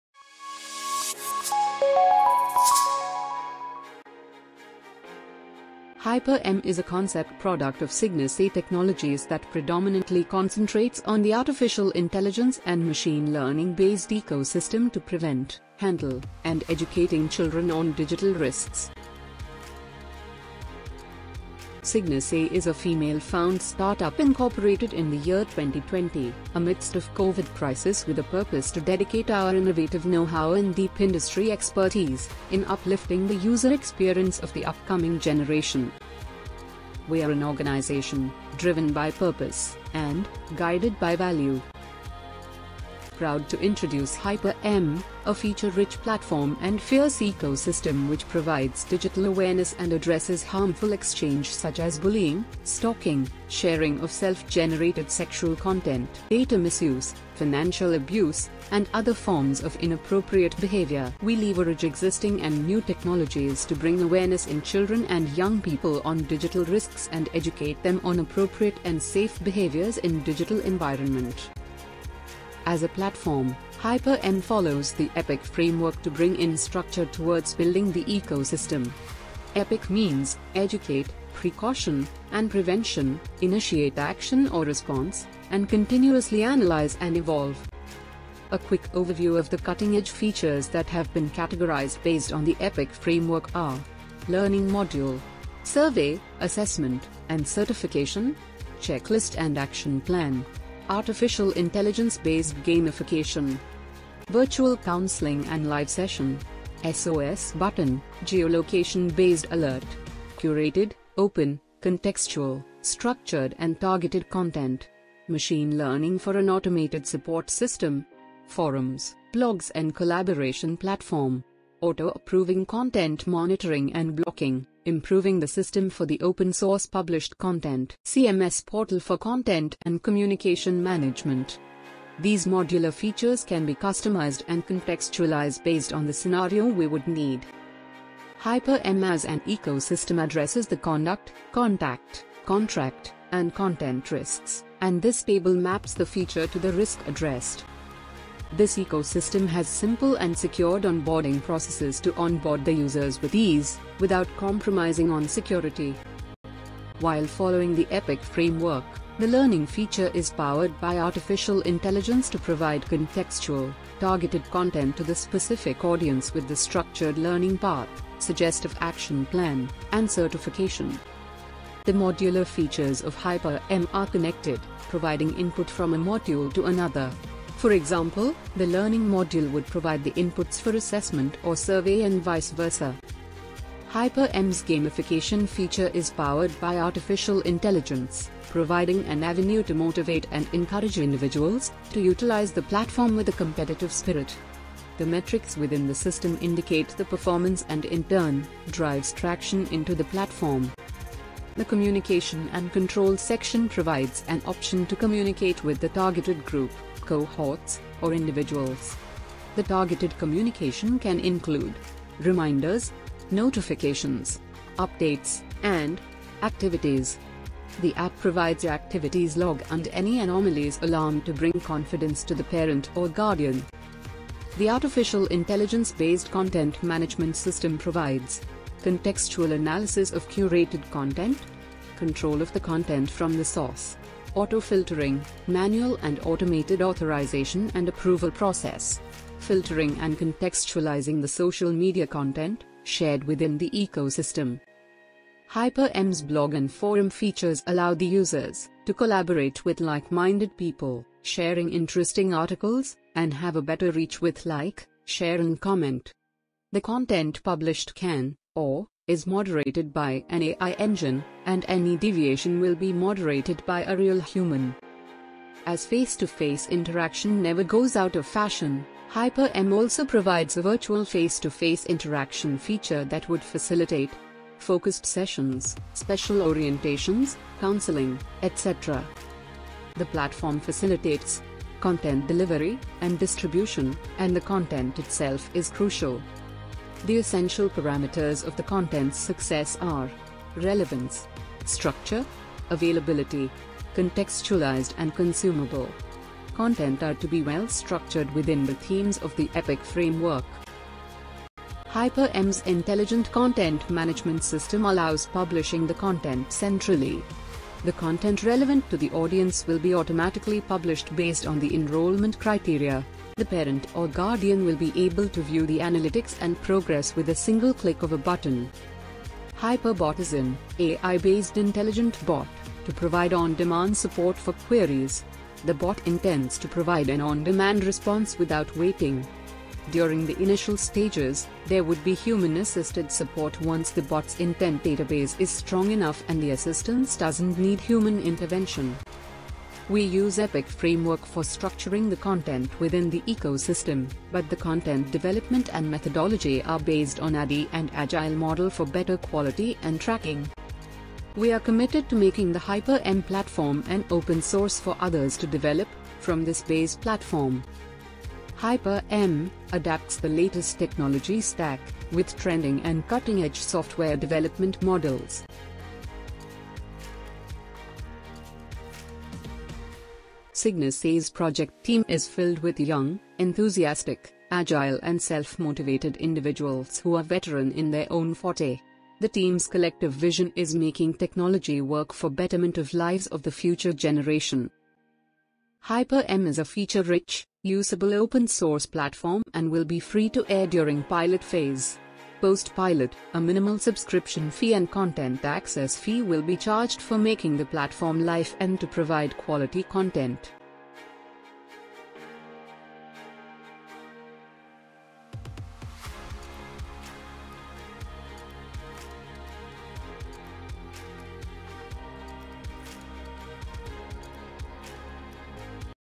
Hyper M Ecosystem
HYPER-M is an innovative hybrid Mobile/Web ecosystem, leveraging existing and new technologies to educate children and young people about digital risks awareness, appropriate and safe behaviors in digital environments.